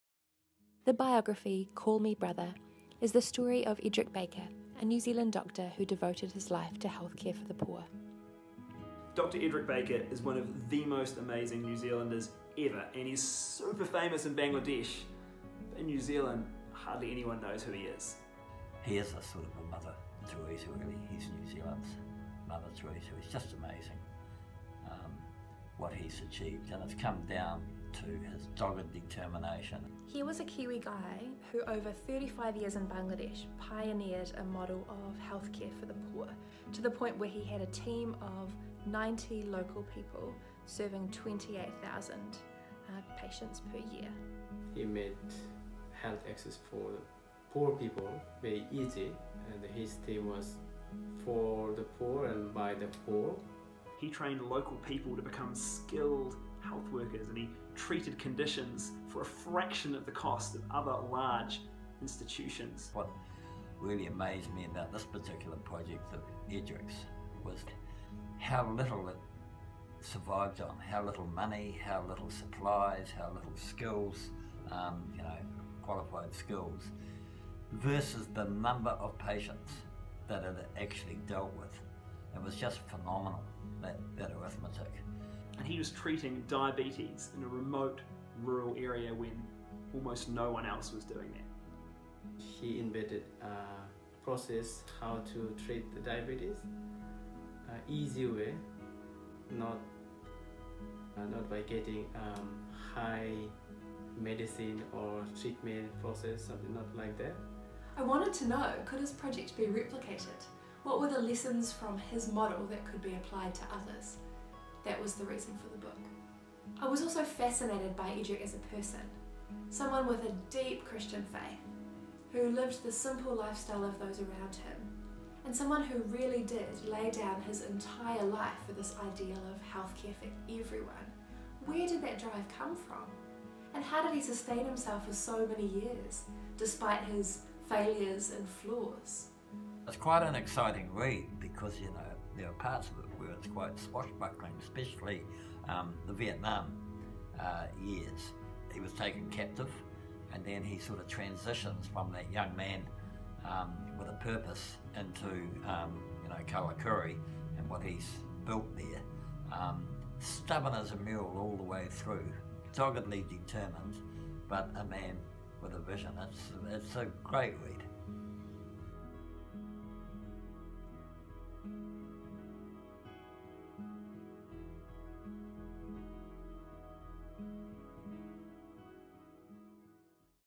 Call Me Brother - Biography of Doctor Edric Baker
'Call Me Brother' is the biography of Edric Baker, a New Zealand doctor who devoted his life to healthcare for the poor.